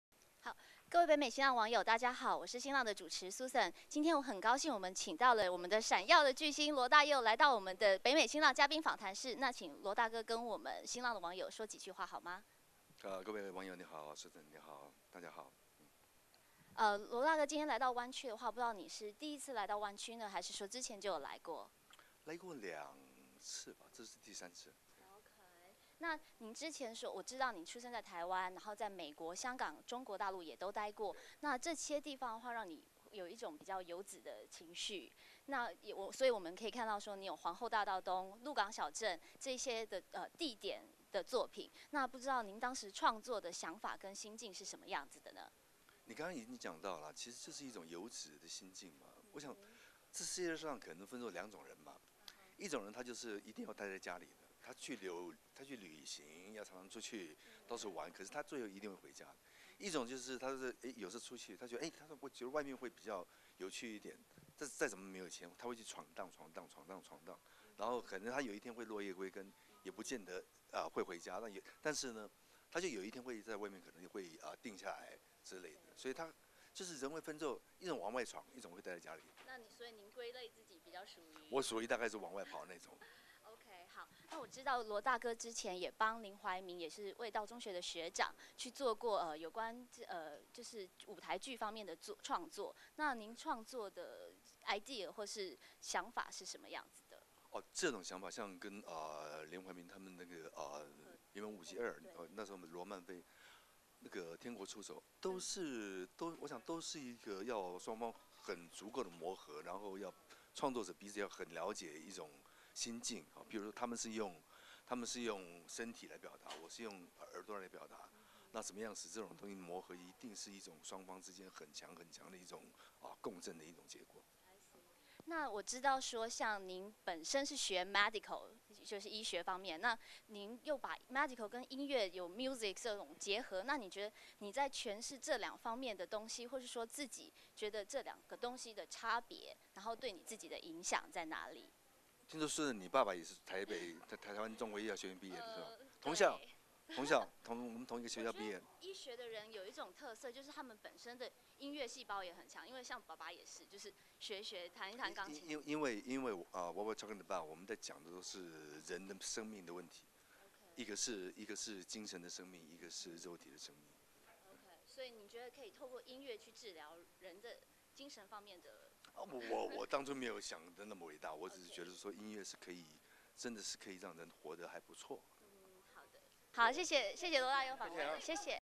北美新浪嘉賓專訪 羅大佑
北加州慶祝馬蕭當選晚會, 並同時為四川大地震受難的四川同胞們募款, 晚會由台灣綜藝界主持一哥 徐乃麟擔當, 表演者請到說唱俱佳的高凌風, 歌聲柔美的趙詠華, 與身為樂壇大哥的羅大佑!! 讓晚會氣氛在充滿熱情和愛心的氣氛中, 圓滿成功!